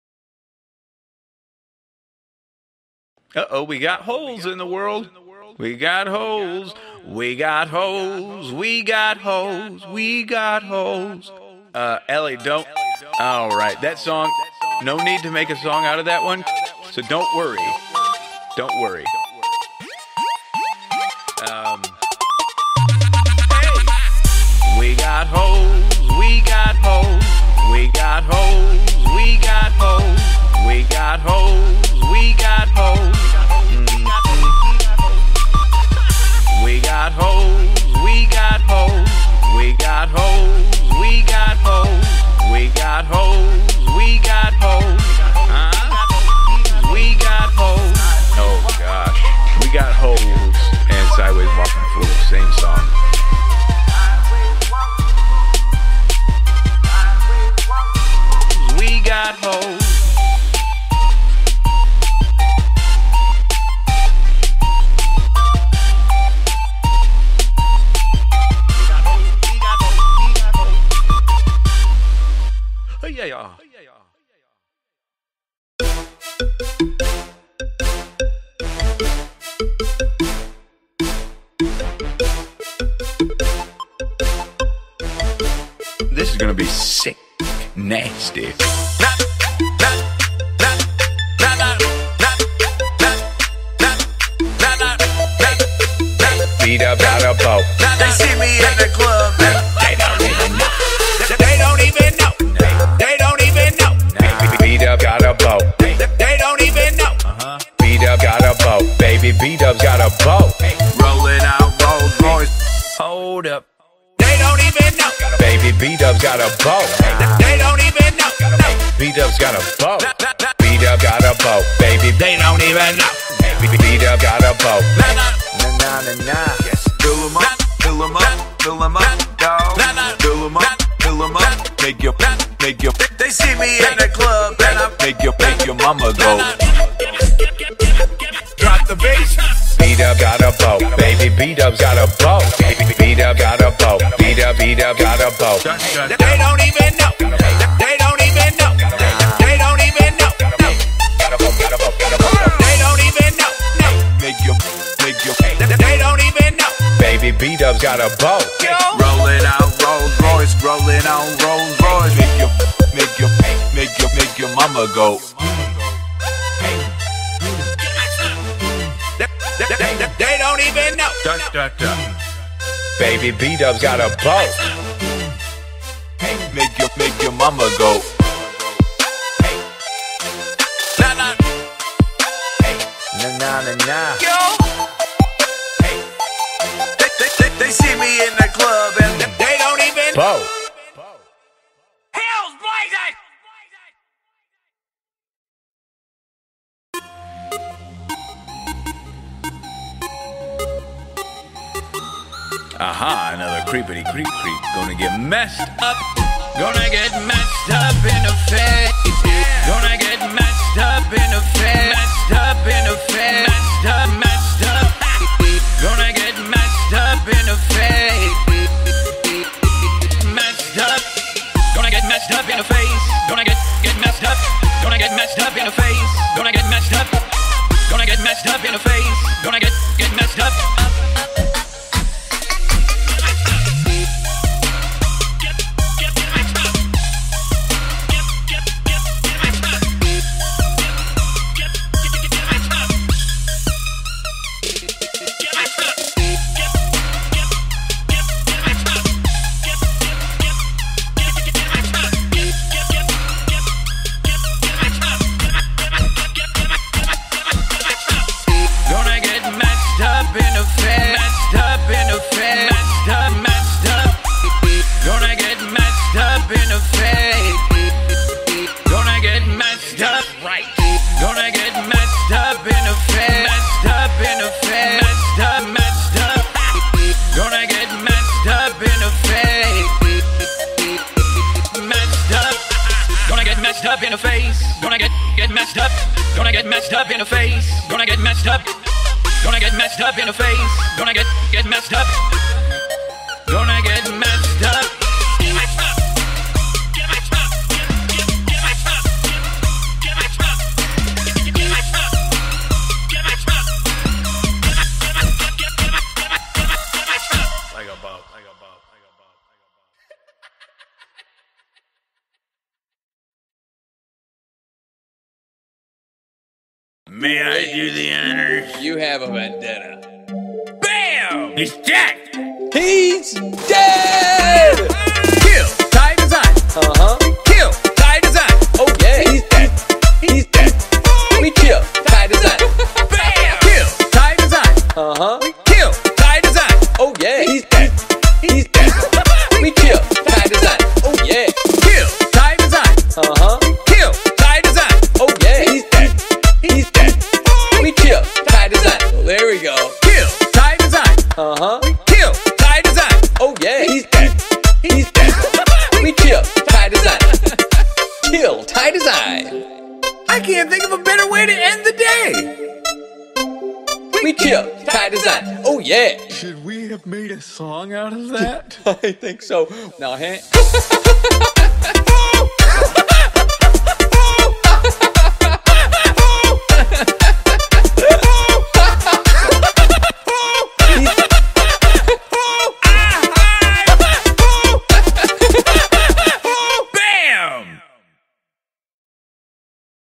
Mini³ - PPS 2015 - Team AC vs. The Suego Stone Picks
The first match is here! Who will move on - Team AC, or The Suego Stone Picks? Which team is ♫ gonna get messed up in the face ♫? ;)

AC

Hasta la Suego

OutdoorJeff
Scheltsy
Pickle Bros

The Suego Stone Picks

Team Variety

This video was created with the ReplayMod, developed by CrushedPixel.

This video was filmed on BenBaptist's Platform Power server.

EDITING:
Video editing was done using Kdenlive, "a free and open-source video editor for GNU/Linux, Mac OS X, and FreeBSD".